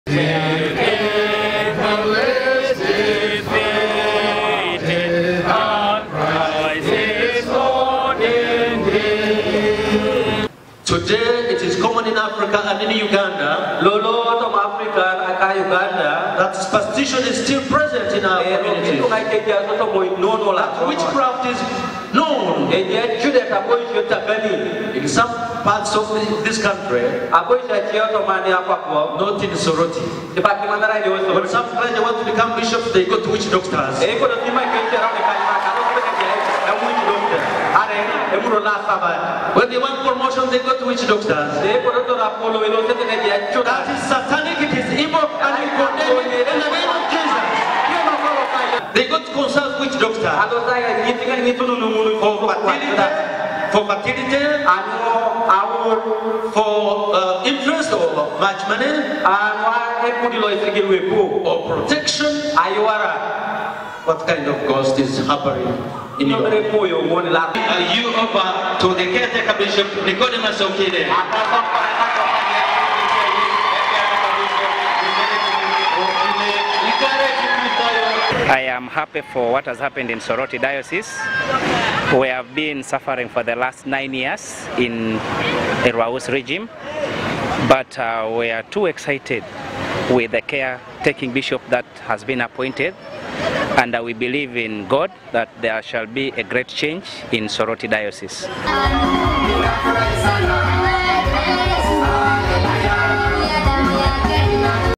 Archbishop Ntagali cautions the clergy against practice of witchcraft
The Arch Bishop of Uganda Stanley Ntagali has warned the clergy and all Christians to desist from practicing witch craft. The Arch Bishop was speaking at St. Peter’s cathedral in Soroti. During the service, Ntagali was expected to announce the name of the new bishop of Soroti diocese to replace Bishop George William Erwau whose tenure ended but he instead announced the position vacant following failure by the house of Bishops sitting in Kampala, to choose an appropriate candidate for the position, citing external influence by the sitting Bishop and some Christians. Ntagali appointed the former Bishop of Bukedi Diocese retired Bishop Nichodemus Okile to care take of Soroti Diocese and also initiate the process of the election of the new Bishop.